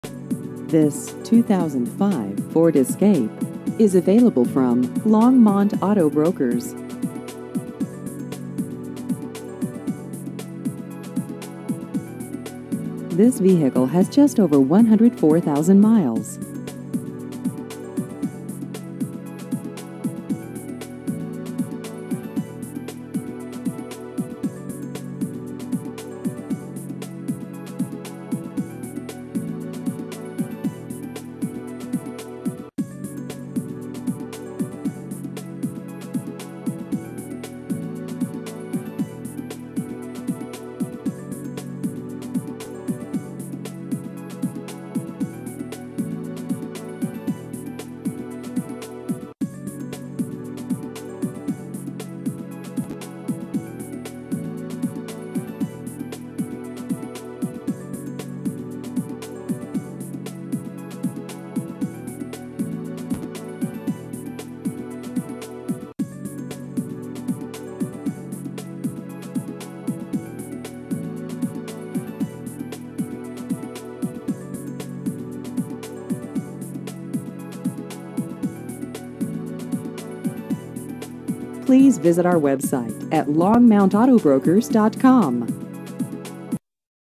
2005 Ford Escape Used Cars Longmont CO
This 2005 Ford Escape is available from Longmont Auto Brokers.